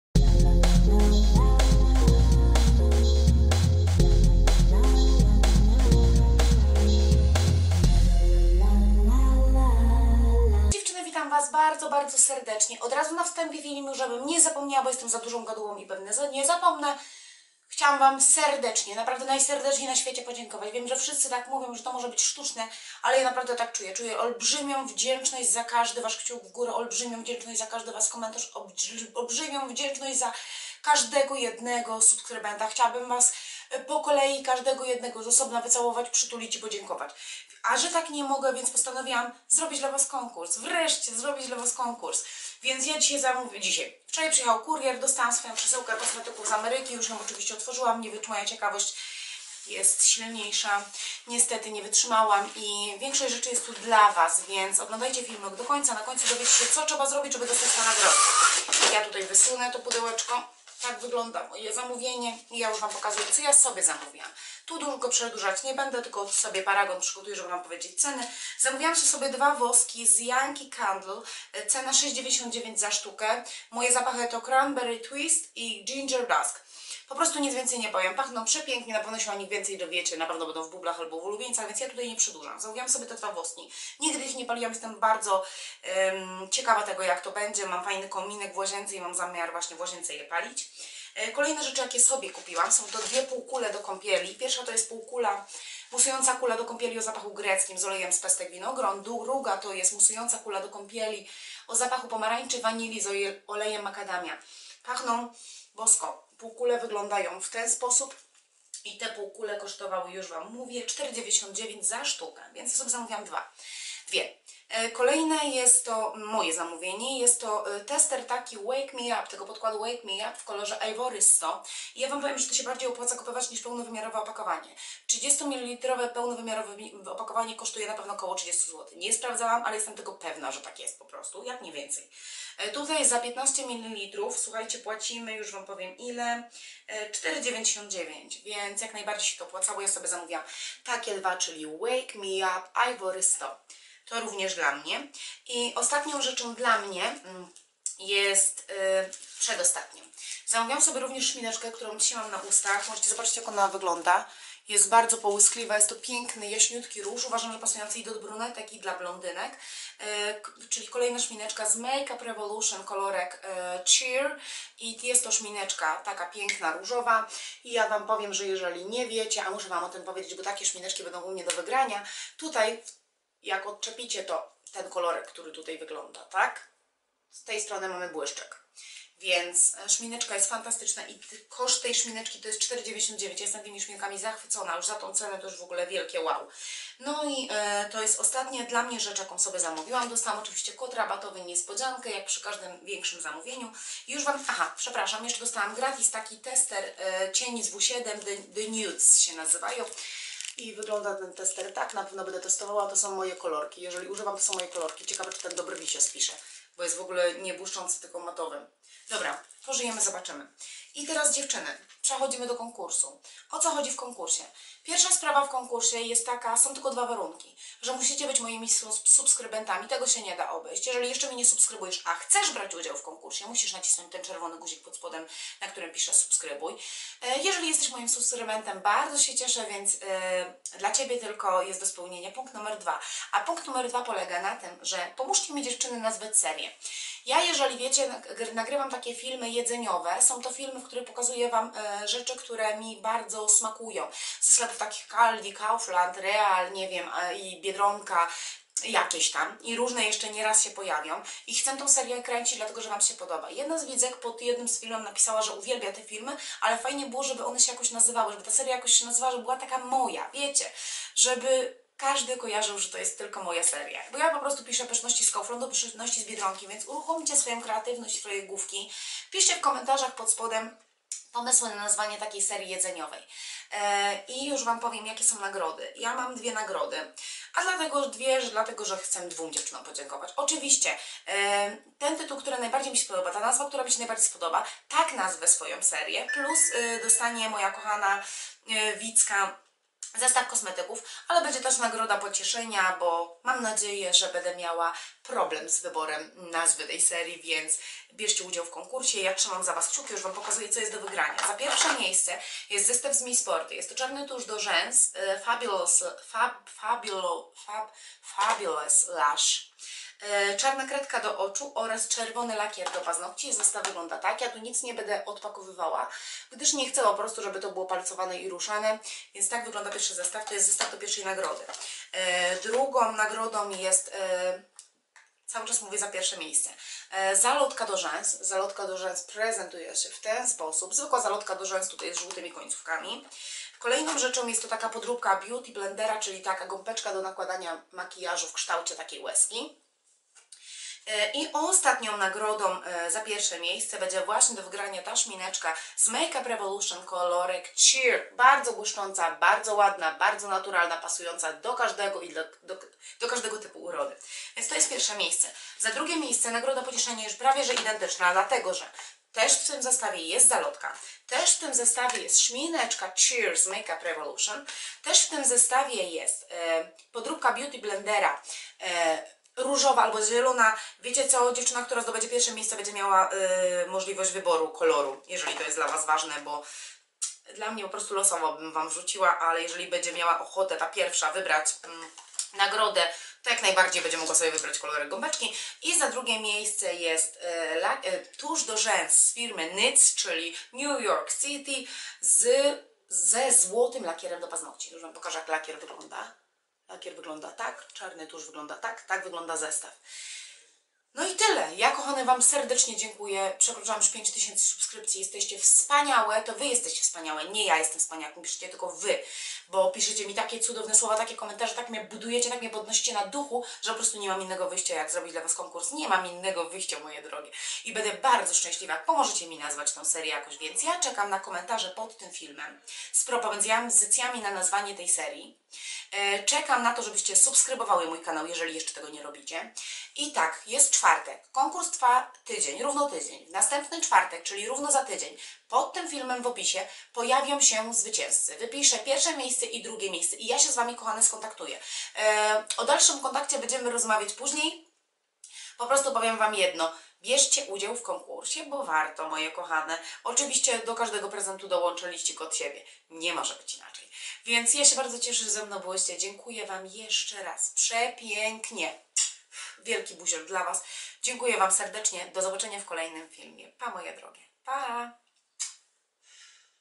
Otwieramy paczkę Kosmetyki z Ameryki # KONKURS# ROZDANIE
DZIĘKUJĘ KOCHANE Z CAŁEGO SERDUSZKA

☠ ZASADY KONKURSU:
1. Czas trwania od 18.02 do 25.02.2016 do godz 20:00
2. W konkursie może wziąć udział osoba dorosła lub niepełnoletnia, posiadająca zgodę rodziców
3. Należy napisać komentarz z propozycją nazwy dla serii moich filmików jedzeniowych
4. Jedna osoba może zostawić jeden komentarz (należy zmieścić się w limicie znaków)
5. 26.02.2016 wybiorę najbardziej kreatywny komentarz i nagrodzę dwie osoby nagrodami pokazanymi w filmie
6. Wyniki pojawią się w opisie pod filmem konkursowym w opisie ( dnia 26.02)
7. Na kontakt ze strony zwycięzcy czekam tydzień.

Kochane Dziękuję z Całego serduszka za tak duże zaangażowanie, kreatywność i ogrom miłych słów
Wybór łatwy nie był i każdej z WAS ślę ogromne buziaki...

KONKURS WYGRYWA
 Marlena C ,, MyśliWIĘC Smakuje''

Ta nazwa serii najbardziej trafiła w moje serce i upodobania

NAGRODA POCIESZENIA
 Sznurasek Bloguje  ,,Smakołyki(NIE) z Ameryki ''

Rozbawiłaś mnie Kochana bardzo tą nazwą